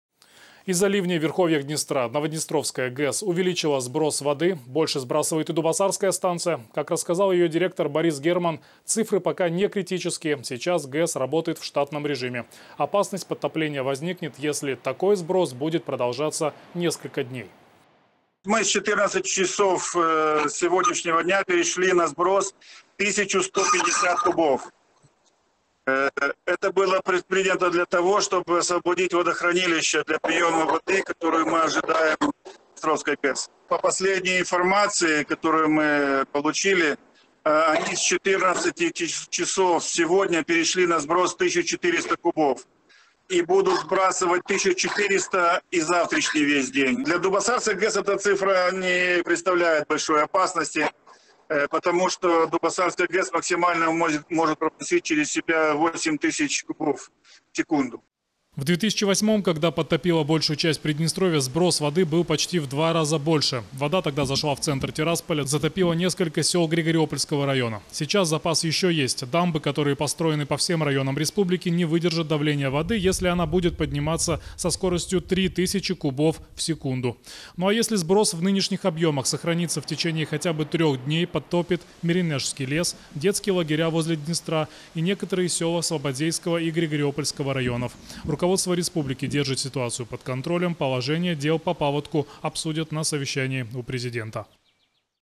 Кому грозит подтопление
Из-за ливней в верховьях Днестра Новоднестровская ГАЭС увеличила сброс воды. Больше сбрасывает и Дубоссарская станция. Как рассказал её директор Борис Герман, цифры пока не критические. Сейчас ГЭС работает в штатном режиме. Опасность подтопления возникнет, если такой сброс будет продолжаться несколько дней.